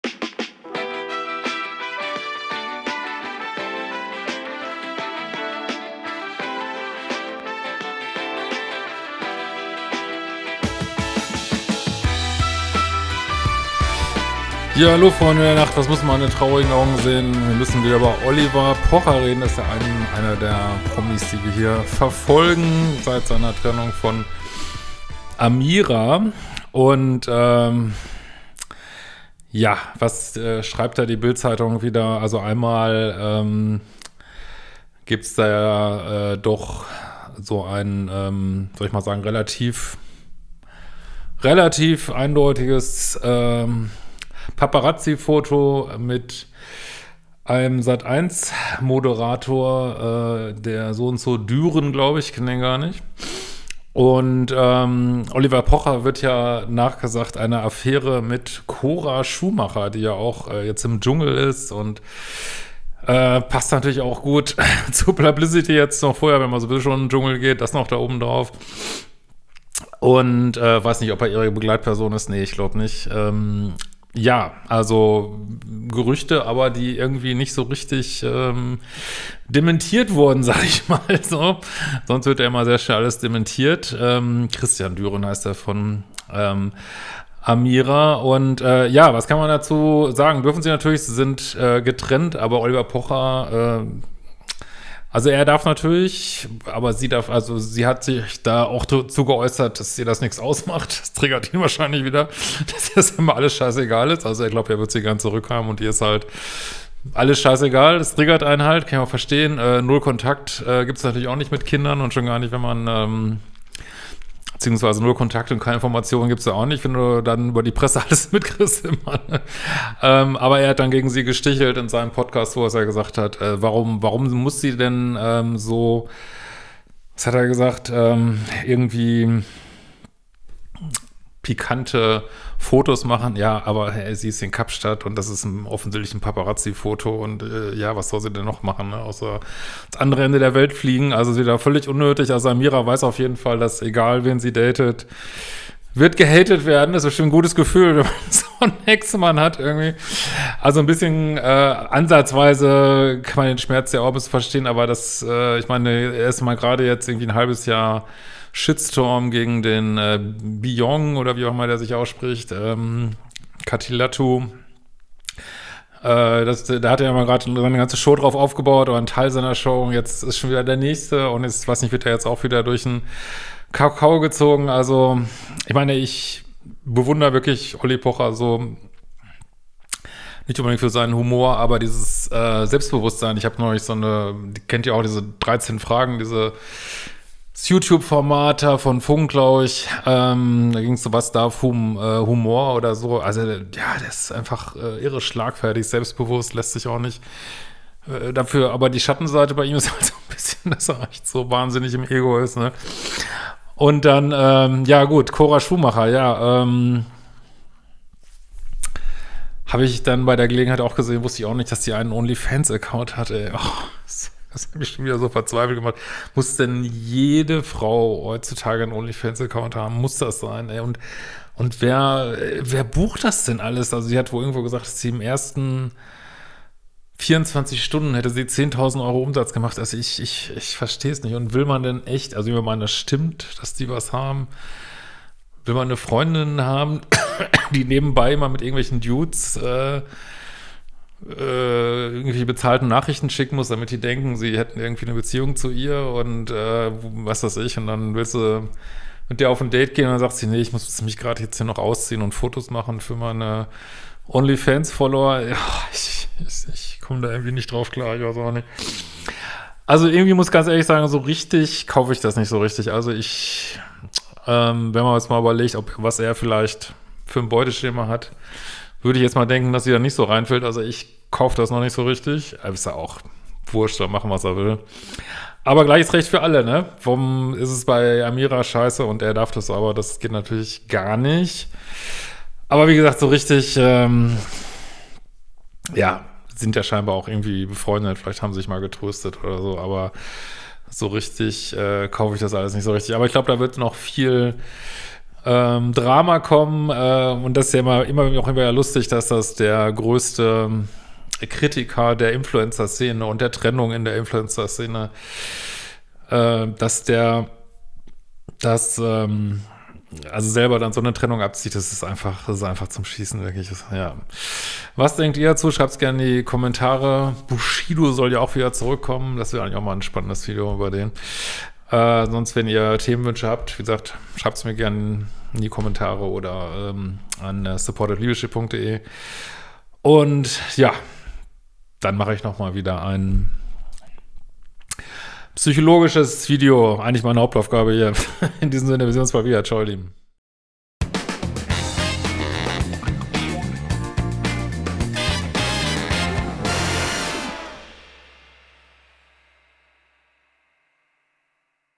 Neue Dramen um Amira & Oli Pocher - Hat Cora Schumacher alles bestätigt?
Mal wieder eine neue Folge der beliebten oliverpocher & amirapocher Serie. In dieser Folge treten überraschen zwei neue Protagonisten auf, passend zum Dschungel.